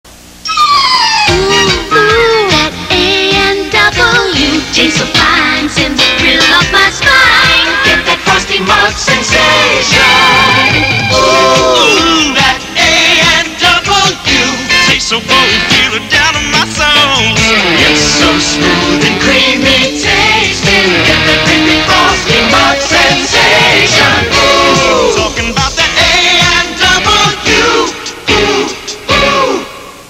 1981 A&W Root Beer commercial. Featuring Patrick Swayze on roller skates!
1981 A&W Root Beer commercial.